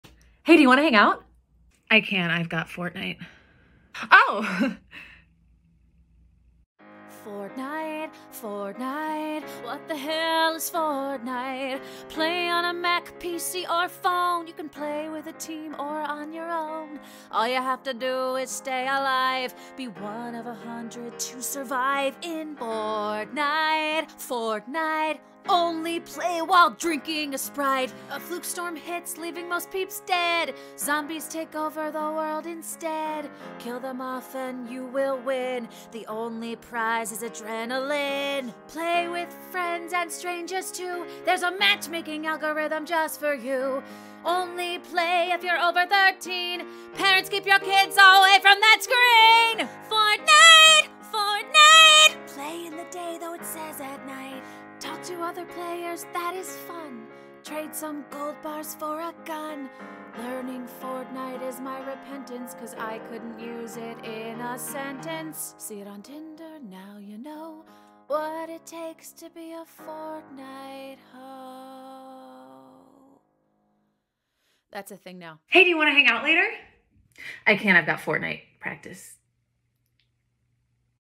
WHAT IS FORTNITE?
Tired of hearing everyone talk about Fortnite and you have no idea what's happening? I will teach you everything you need to know and in song! Impress your friends, impress your date, impress yourself. Or...just play Candy Crush.